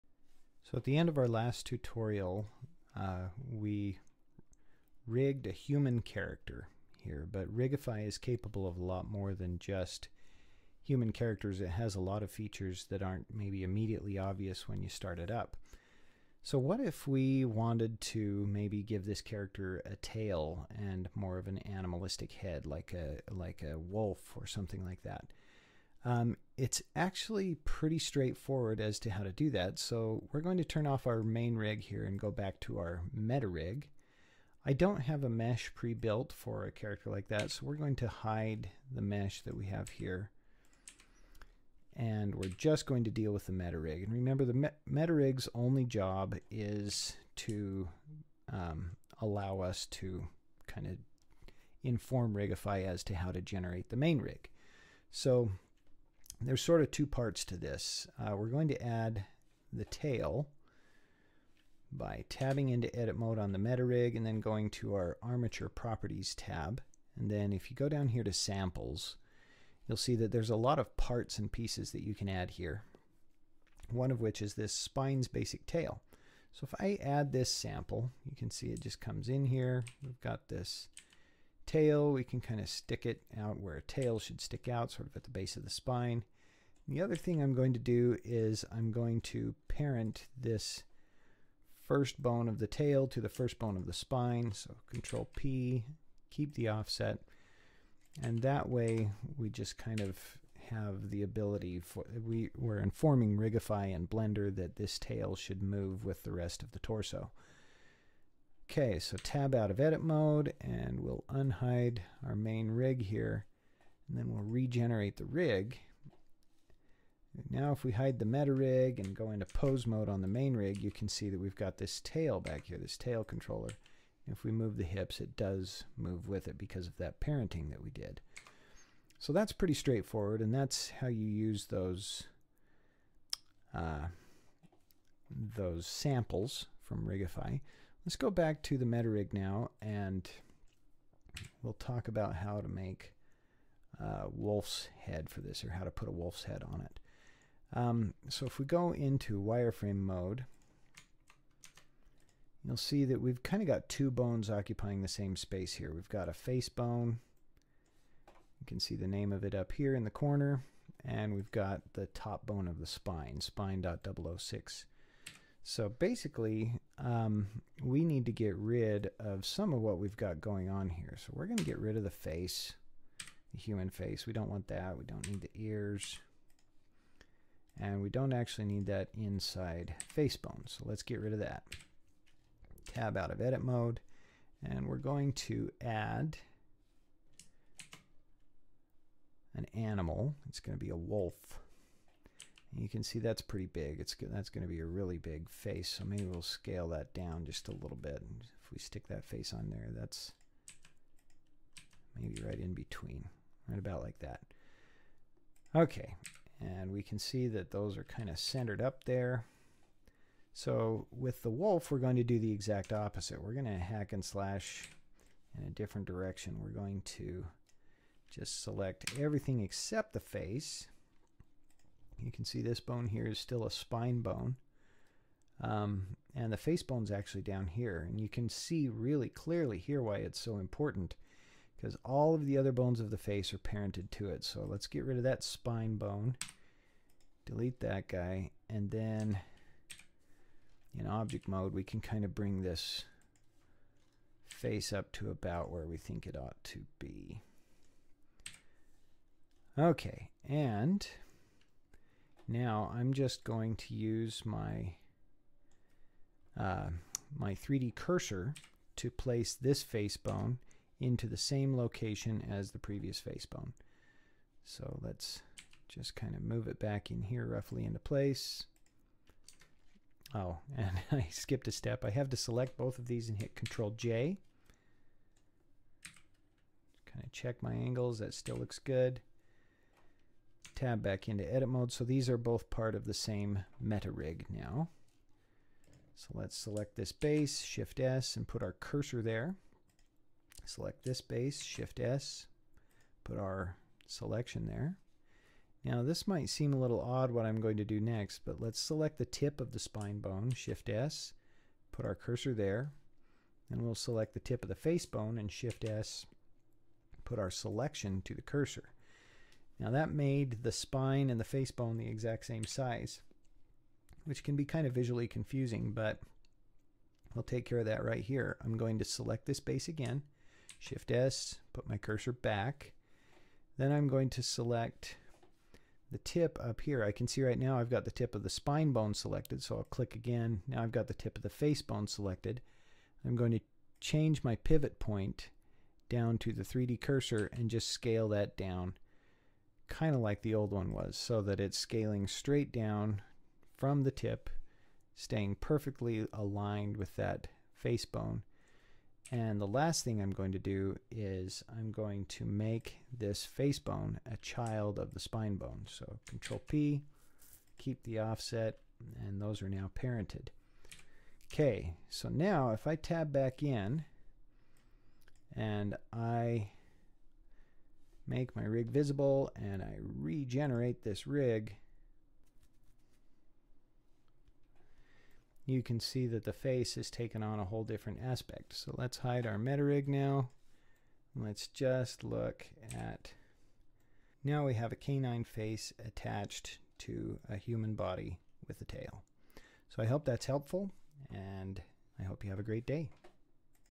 Blender Tutorial 38 - Using Rigify to rig non-human characters
Topics covered:
Creating a tail using rigify samples
Replacing a human face with a wolf face